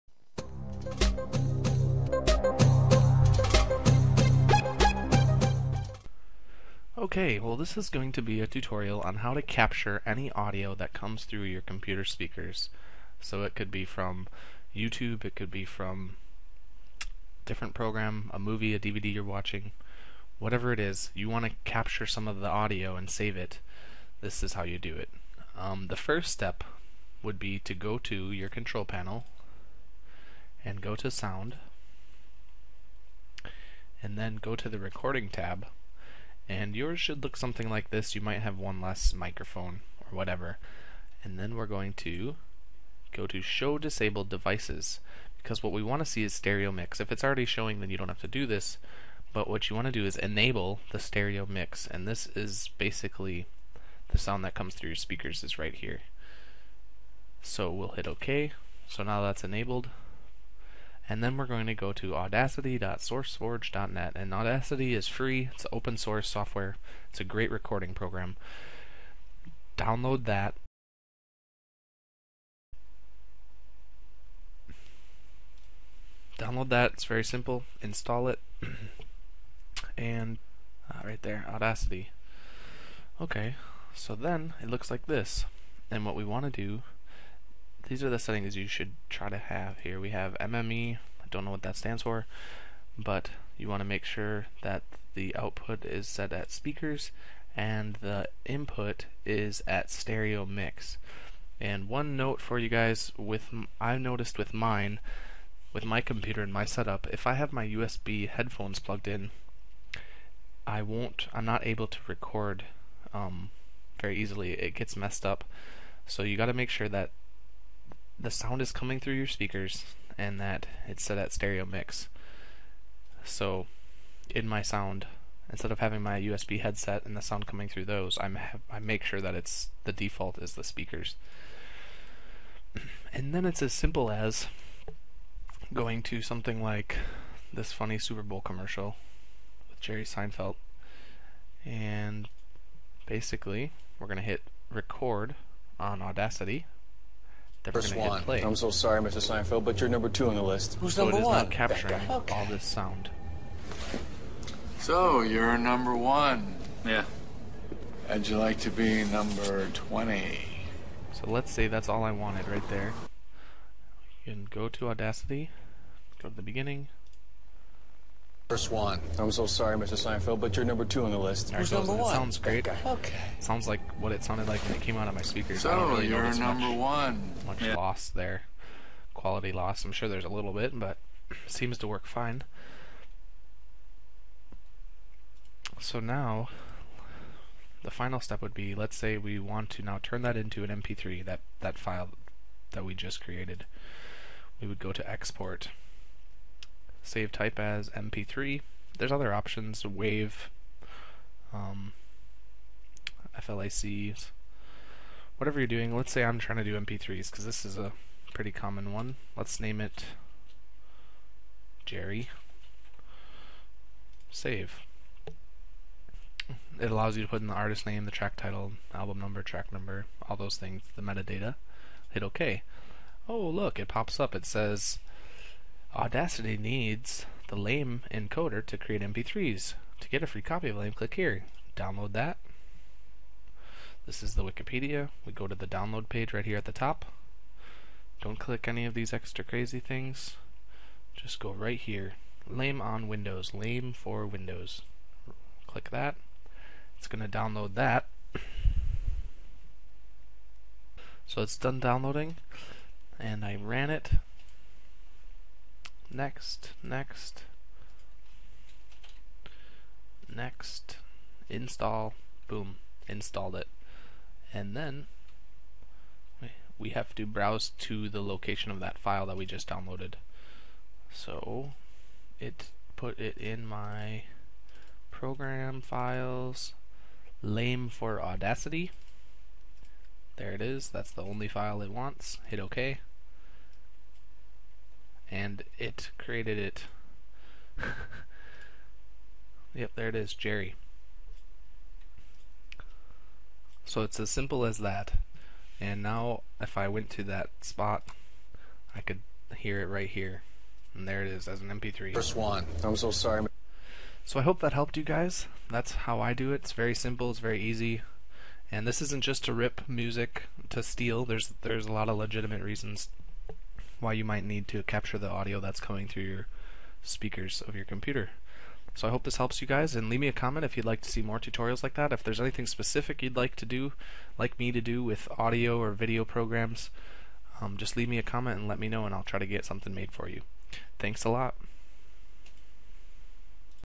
How to: Record/Save Audio from ANY Source - Audacity Tutorial -
This is how I personally record and save audio from any source on my computer, including Youtube, personal DVD's, TV series, Etc.